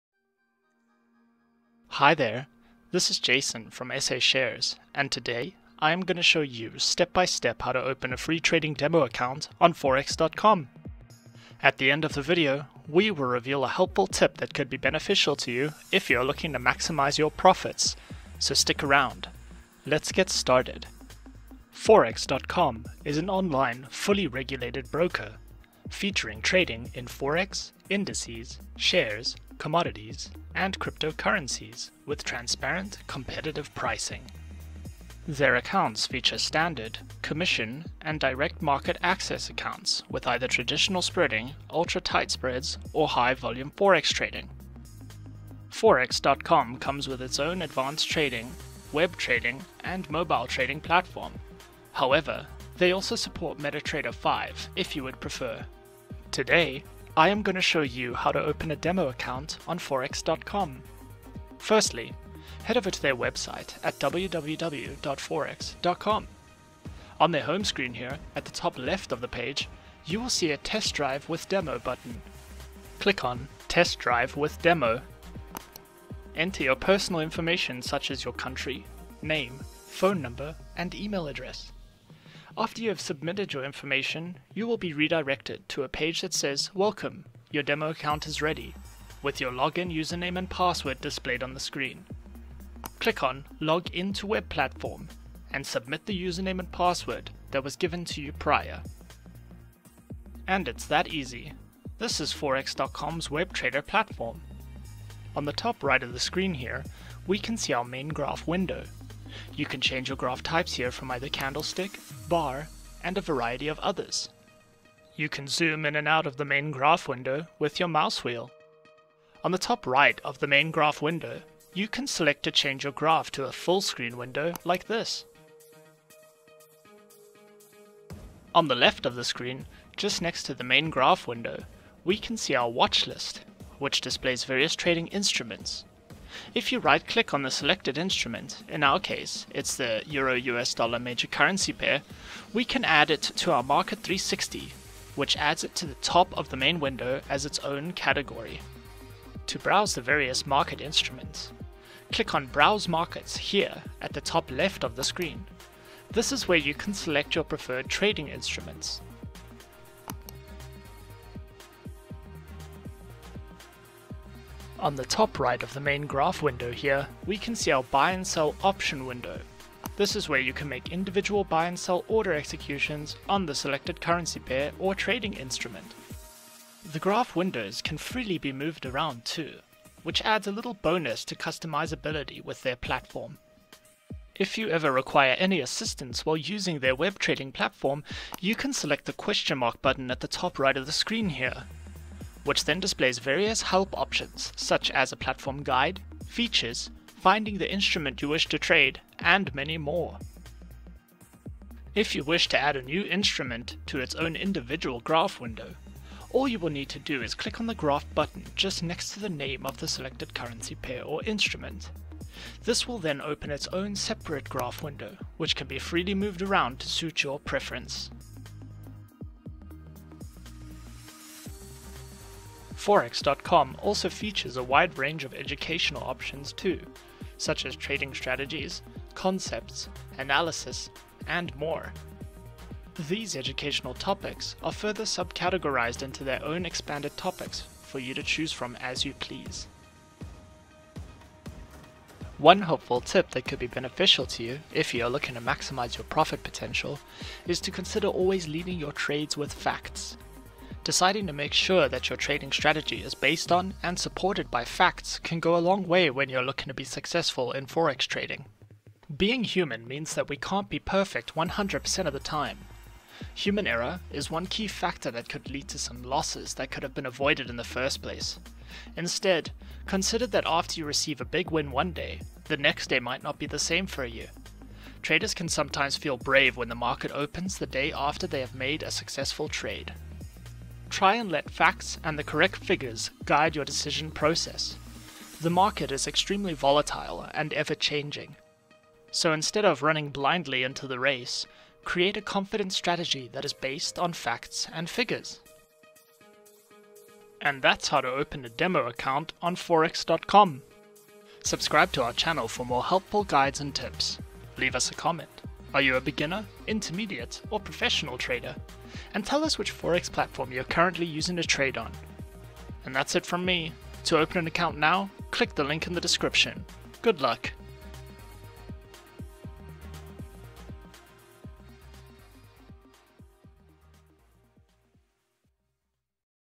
How to Open a Forex.com Demo Account - A Step By Step Guide for Beginners 🔎
In this 5 minute guide, we will show you step by step how to open a FREE Forex.com Demo Account.

This Forex.com Video Tutorial Includes:

📢 Walkthrough the trading dashboard.
📢 How to set up your first trading positions.
📢 How to make your first deposit.
📢 History of the broker.
📢 and more...

Read our Forex.com Video Script:

Hi there! This is Jason from SA shares, and today I am going to show you step by step how to open a free trading demo account on Forex.com

At the end of the video, we will reveal a helpful tip that could be beneficial to you if you are looking to maximize your profits, so stick around. Let’s get started

Forex.com is an online, fully regulated broker. Featuring trading in Forex, indices, shares, commodities and cryptocurrencies with transparent, competitive pricing.

Their accounts feature standard, commission, and direct market access accounts with either traditional spread, ultra-tight spread, or high-volume forex trading.

Forex.com comes with its own advanced trading, web trading, and mobile trading platform, however they also support meta trader 5 if you would prefer.

Today, I am going to show you how to open a demo account on forex.com

On their home screen here on the top left, you will see a test drive with a demo button.

After you have submitted your information you will be directed to a page that says welcome, your demo account is ready with your login username and password.

Click on log in to web platform

Submit the username and password that was given to you prior

And its that easy, this is forex.com’s webtrader platform.

On the top right of the screen here we can see our main graph window, you can change your graph types here from either candlestick, bar, and a variety of others.

You can zoom in and out of the main graph window with your mouse wheel.

On the top right of the main graph window, you can select to change your graph to a full-screen window like this

On the left of the screen just next to the main graph window we can see our watchlist which displays various trading instruments if you right-click on the selected instrument in our case it’s the EUR/USD major currency pair we can add it to our market 360 which adds it to the top of the main window as its own category market.

To browse the various market instruments, click on browse markets here at the top left of the screen this is where you can select your preferred trading instruments

On the top right of the main graph window here we can see our buy and sell option window this is where you can make individual buy and sell order executions on the selected currency pair or trading instrument.

The graph window can freely be moved around too, which adds a little bonus to customizability with their platform.

If you ever require any assistance while using their web trader platform you can select the question mark button on the top right of the screen here which then displays various help options such as a platform guide, features, finding the instrument you wish to trade, and many more.

If you wish to add a new instrument to its own individual graph window all you will need to do is click on the graph button just next to the name of the selected currency pair or instrument.

This will then open its own separate graph window which can be freely moved around to suit your preference.

Forex.com also features a wide range of educational options too, such as trading strategies, concepts, analysis, and more.

These educational topics are further subcategorized into their own expanded topics for you to choose from as you please.

One helpful tip that could be beneficial to you if you are looking to maximize your profit potential is to consider always leading your trades with facts – deciding to make sure that your trading strategy is based on and supported by facts can go a long way when you are looking to be successful in Forex trading.

Being human means that we cannot be perfect one hundred percent of the time, human error is a one key factor that could lead to some losses that could have been avoided. Instead, consider that after you receive a big win one day, the next day might not be the same for you. Traders can sometimes feel brave when the market opens the day after they have made a successful trade.

And that’s how to open a demo account on Forex.com  – subscribe to our channel for more helpful guides and tips – Leave us a comment: Are you a beginner / Intermediate or Professional trader - and tell us which forex platform you are currently using to trade on.